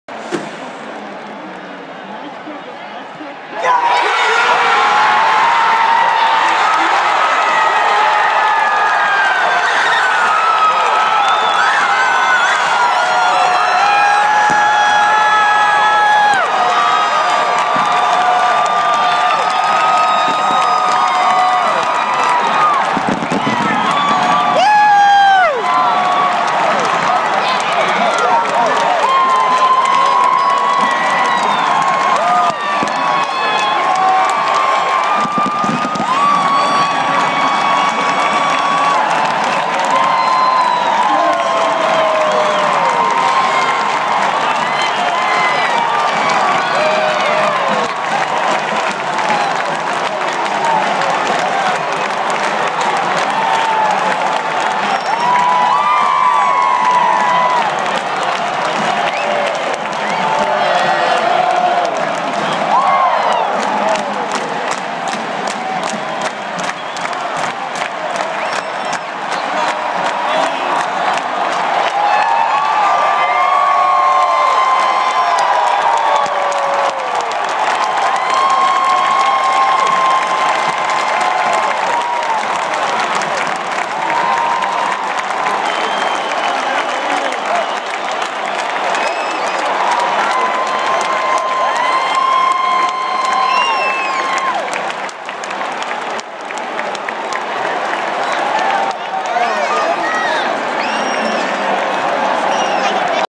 Palardy's winning field goal
Game winning field goal at UT vs South Carolina